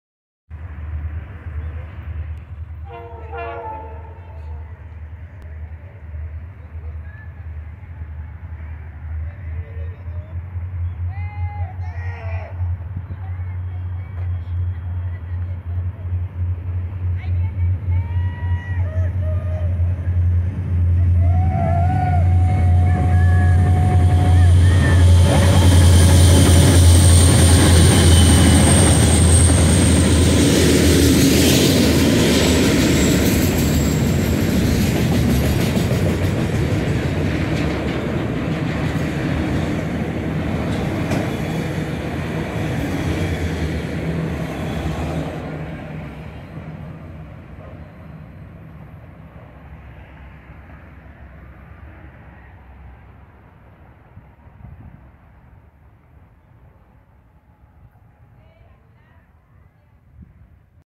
2 Viejitas, FerroCoca & 🍉 en un MICRO AUTOMOTRIZ LOCAL de FXE N° 3171 GMXT & 3224 Dual SD-40-2 MX©®
Tren de Carga con 2 Locomotoras:
Ferrocarril Mexicano (Ferromex/FXE) GRUPO MÉXICO TRANPORTES (GMXT) FerroCoca N° 3171 SD-40-2  Electro-Motive Diesel (EMD).
Ferrocarril Mexicano (Ferromex/FXE) N° 3224 SD-40-2 Electro-Motive Diesel (EMD).
Con 08 Vagones/Carros.
Tipo de Vagones/Carros: Autorack Bi-Nivel.
Día: Viernes 14 de Octubre del 2022.
Ubicación: Avenida Manuel Gómez Morín Casi Esquina con Avenida Alameda a un Costado del Parque de las 3 Centurias en Aguascalientes Aguascalientes, México.